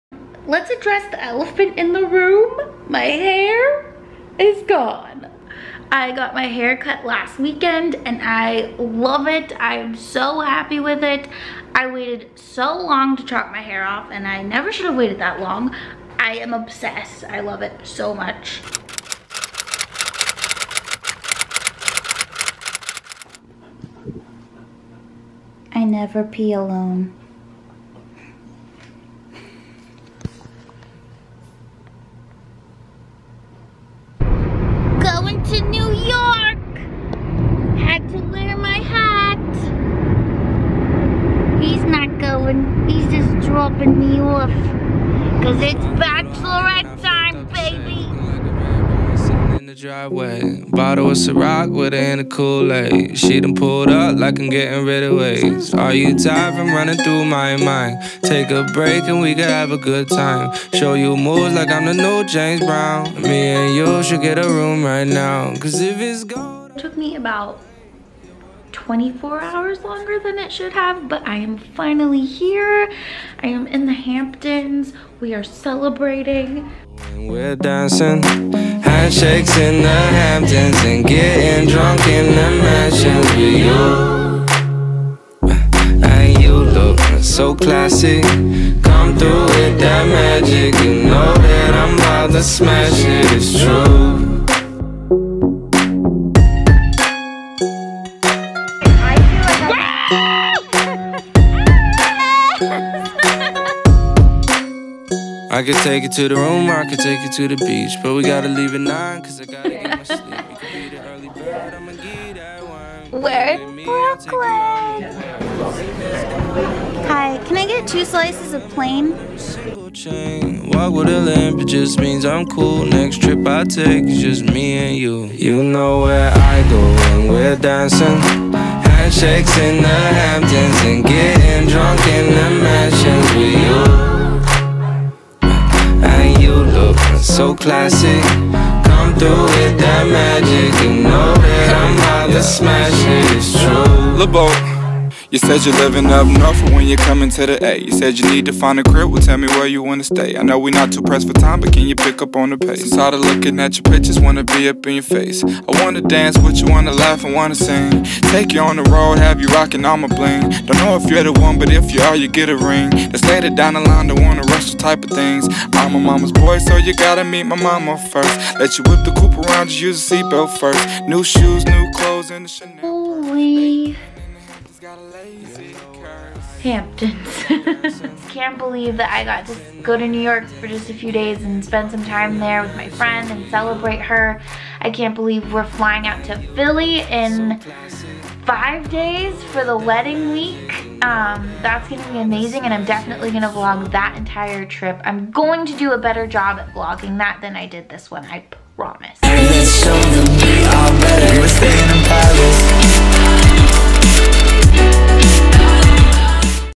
THE HAMPTONS
Celebrated my sweet friend-- the bride to be! Follow along for the recap!

Xx,
Taylor